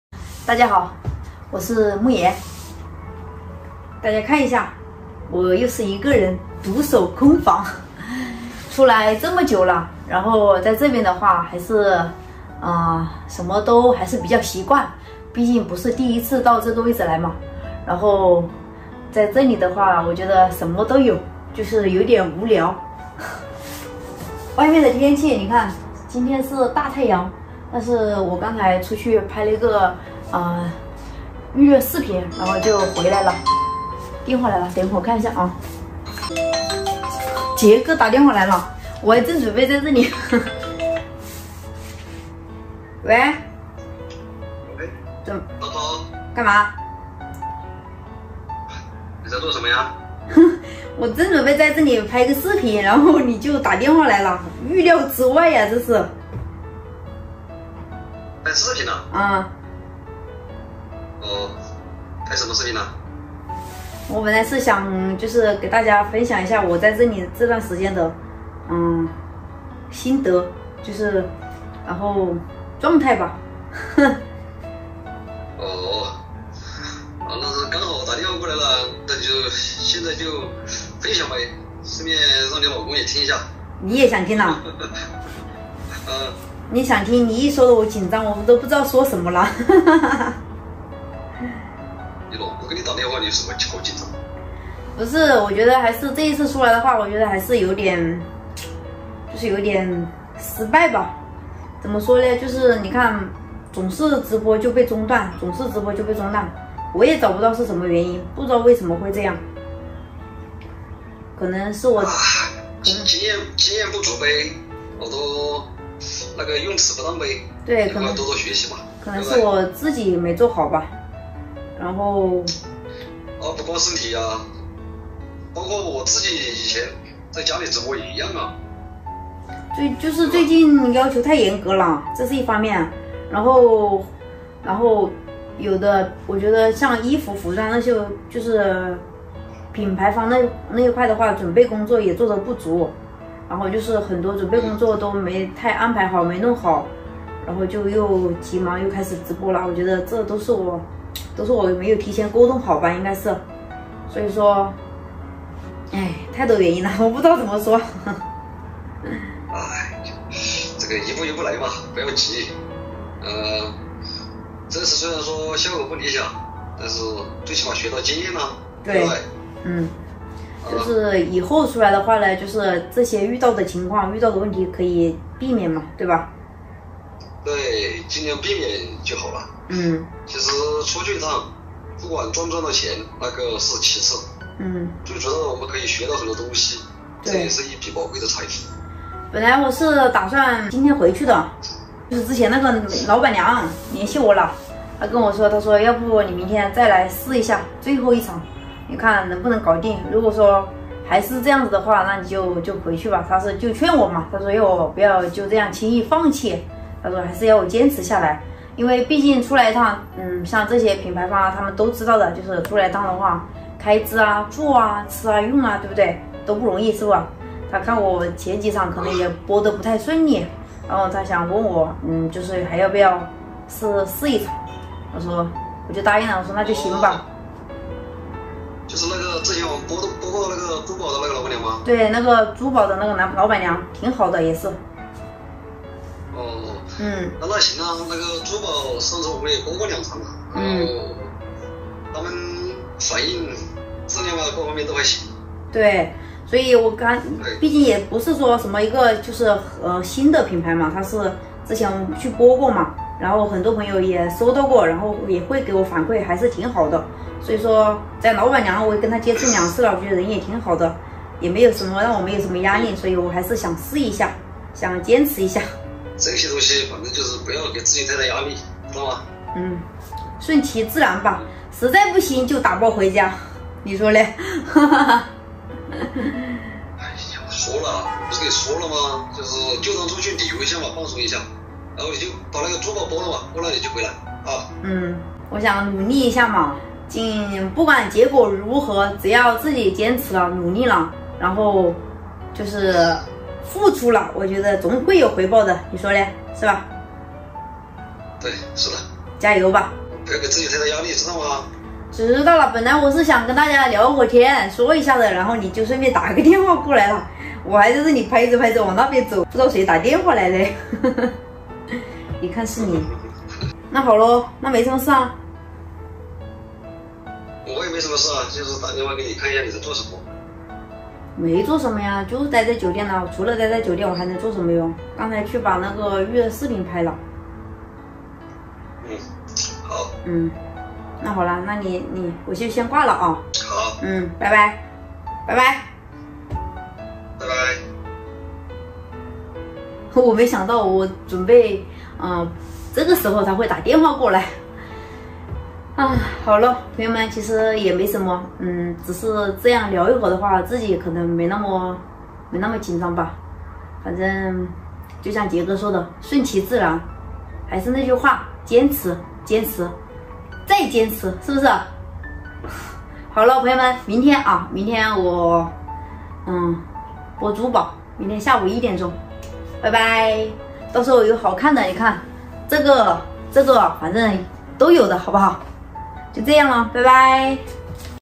夢妍原本打算回家，但是一個老熟人的電話，又讓她改變了行程#農村
请放心，我们的视频旨在教育如何安全、合法地处理害虫问题，无任何动物受害。 哈嘍~大家好，這裡是【土家阿傑】YouTube官方正版頻道